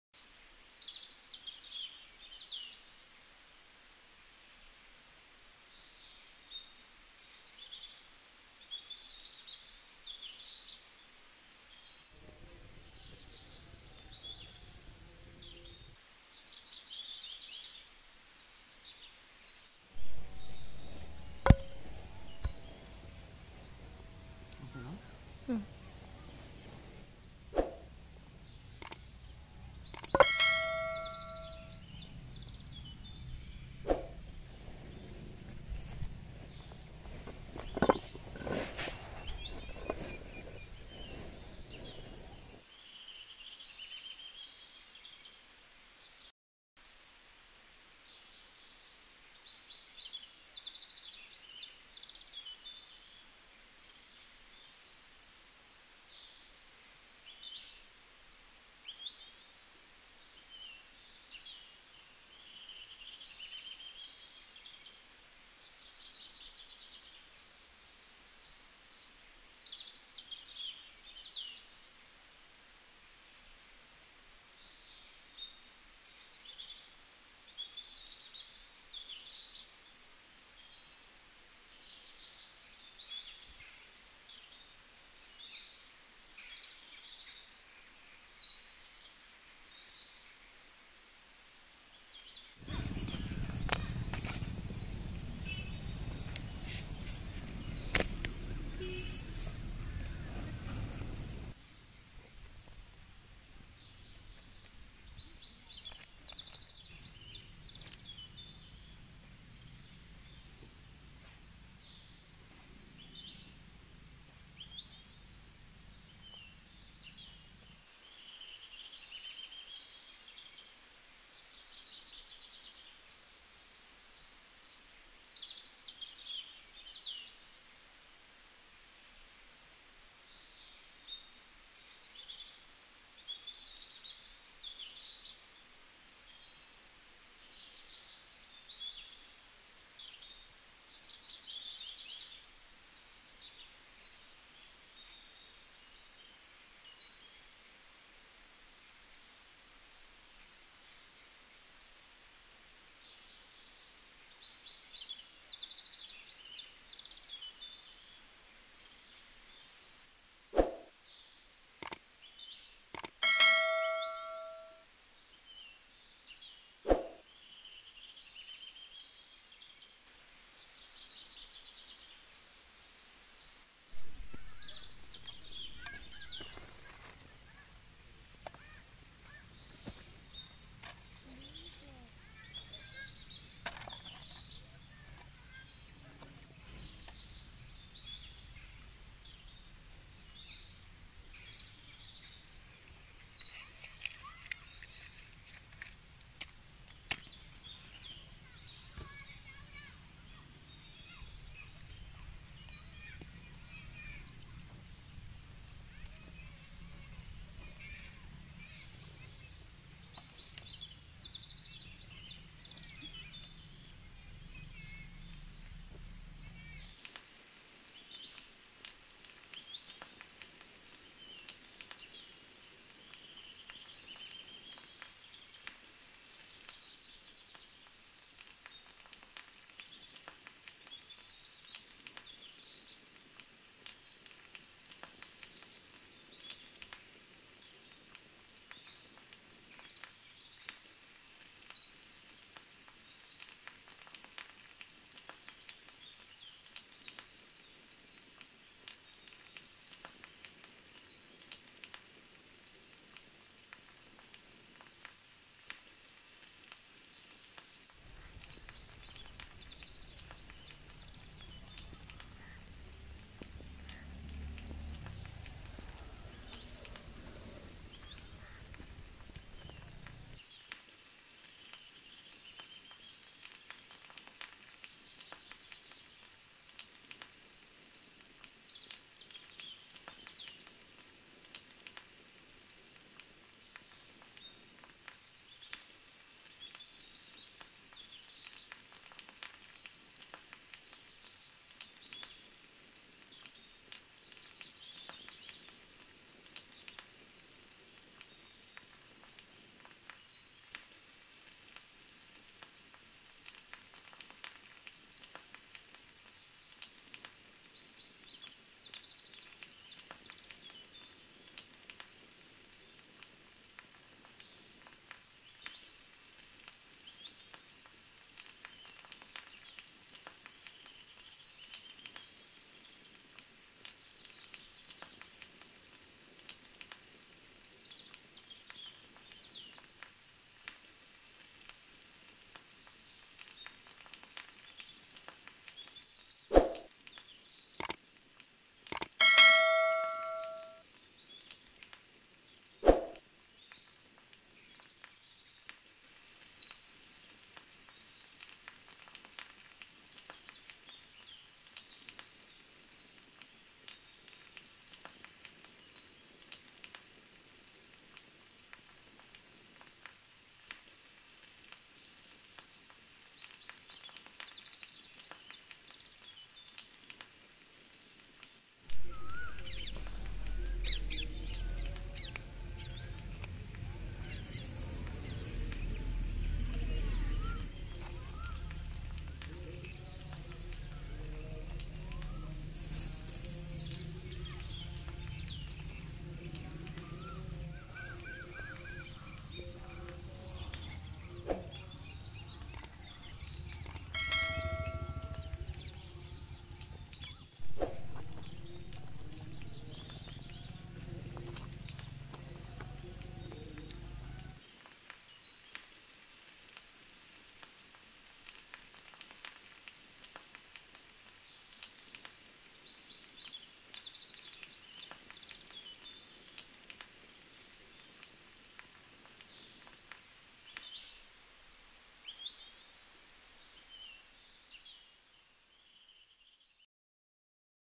Barbecue Nation Style Prawns at Home | Very Simple| Outside Cooking- Ma Village Spices
Barbecue Style Prawns | Very Simple| Outside Cooking

Best Grilled Prawn Ever - Outside Cooking| Close To Nature

Ingredients
Prawns : 250 grms
Red Chilli Powder : 1Tbsp
Salt: 1Tbsp
Garam Masala/Prawn Masala:1tbsp
Vegtable Oil: 1Tbsp
GingerGarlic Paste:1Tbsp
Leamon:1
Turmaric Powder:1/4Tbsp